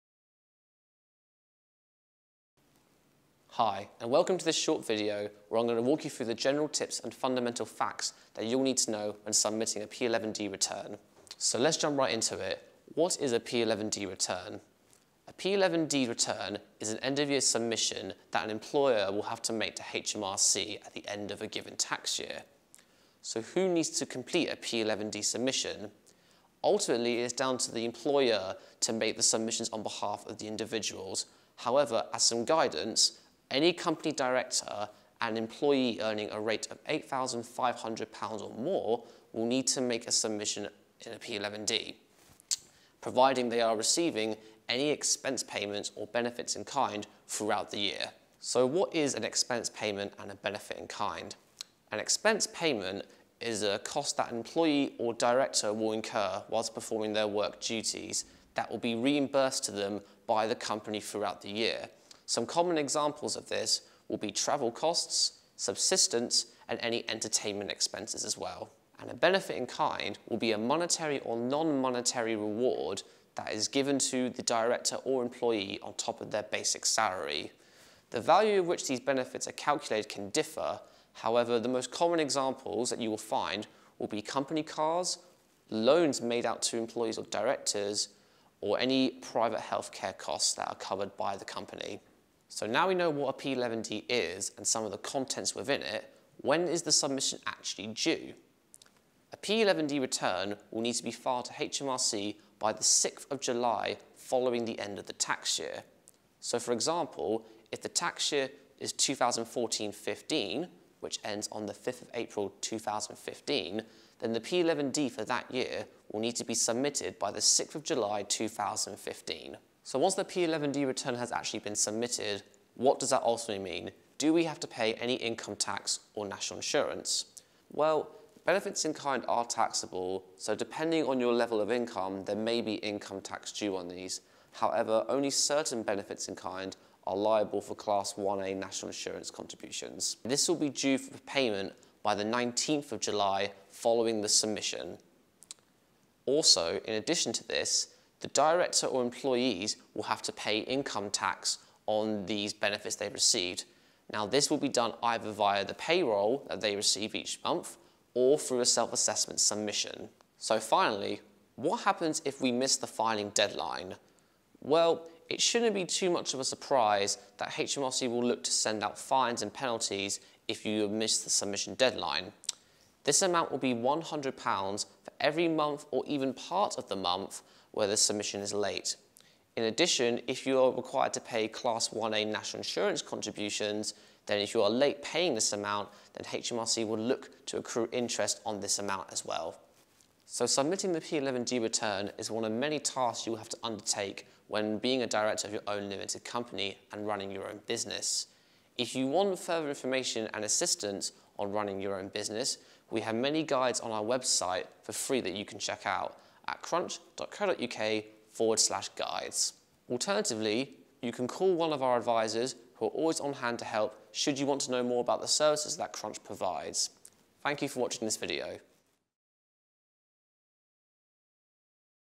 What is P11D? | Crunch
Need more info?

In this video, we explain what a P11D form is meant to do, what’s on it, when you need to file it, and what will happen if you don’t!

Crunch provides accountancy software specifically designed for freelancers, contractors and small businesses - with built-in experts to help you manage your business finances.